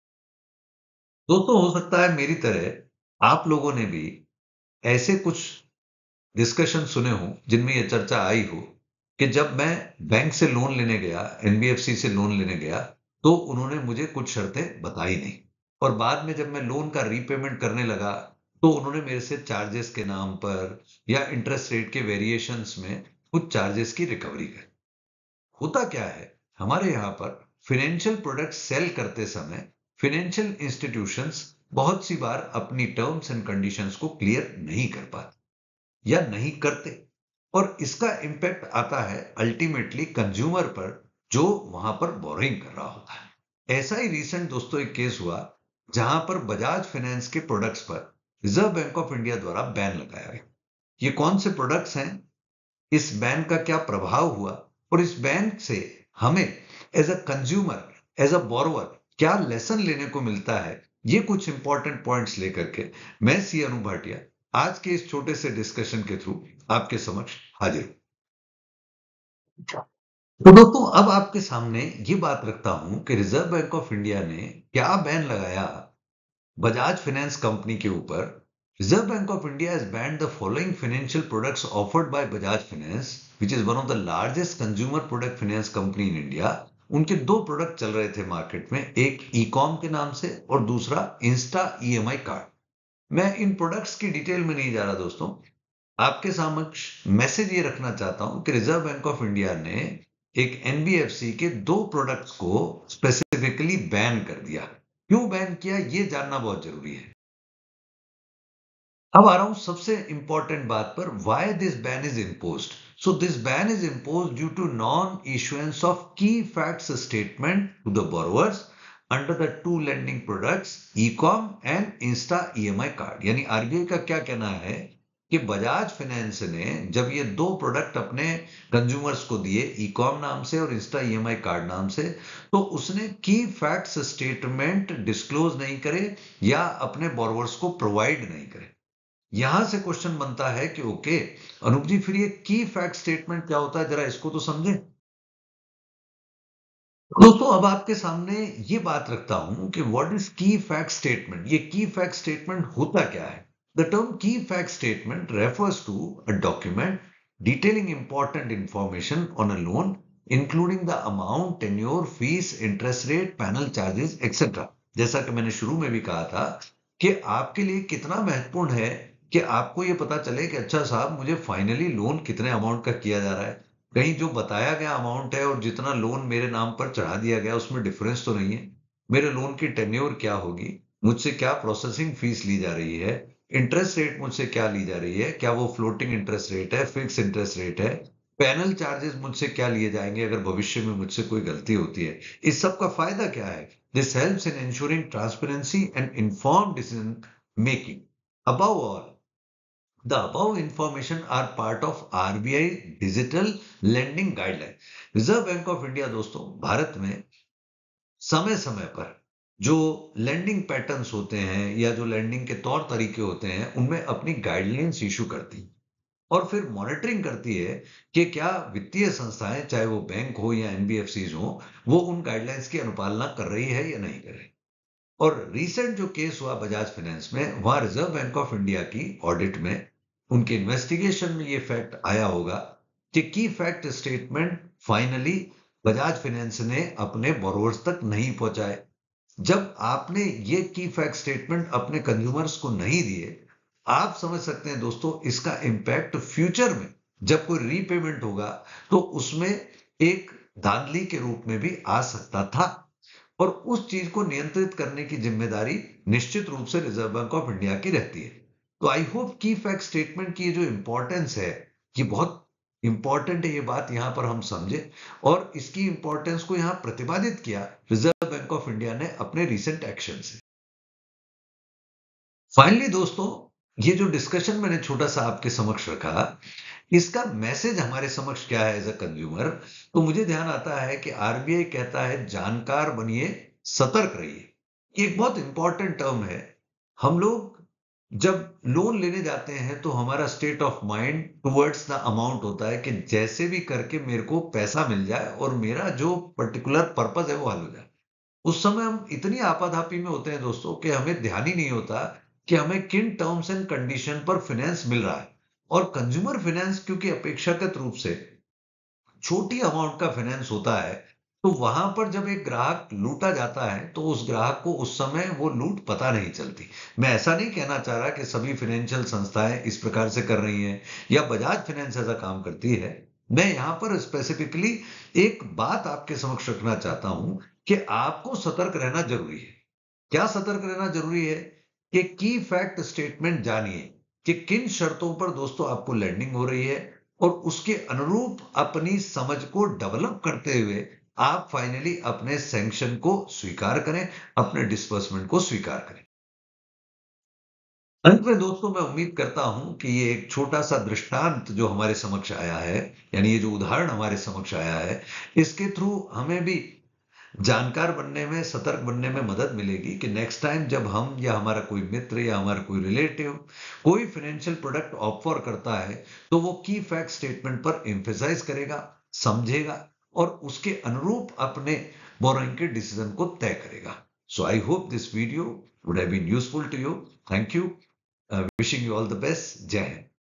RBI द्वारा बजाज फाइनेंस पर लगाए गए बैन से हमें क्या सीखना है/ Lesson from RBI's Ban on Bajaj Finance
नमस्कार दोस्तों, हाल ही में रिज़र्व बैंक ऑफ़ इंडिया (RBI) द्वारा एक गैर बैंकिंग फाइनेंस कंपनी (NBFC) M/s बजाज फाइनेंस के दो फाईनेंशिअल प्रोडक्ट्स (Financial Products) पर बैन लगाया गया है, यह बैन क्यों लगाया गया एवं इसका एक उपभोक्ता अथवा Borrower के रूप में हमसे क्या लेना-देना है, इसी महत्वपूर्ण विषय पर यह वीडियो आपकी सेवा में प्रस्तुत है । आशा करता हूँ आप इस वीडियो को उपयोगी पाएंगे, आपके बहुमूल्य सुझाव एवं प्रश्न सादर आमंत्रित हैं।

Dear Friends, recently the Reserve Bank of India (RBI) has applied ban on two financial products offered by a NBFC called Bajaj Finance. This video explains the key reason for such ban and it’s important message for the borrowers even. I hope you will like the content. Your precious comments and questions are most welcome.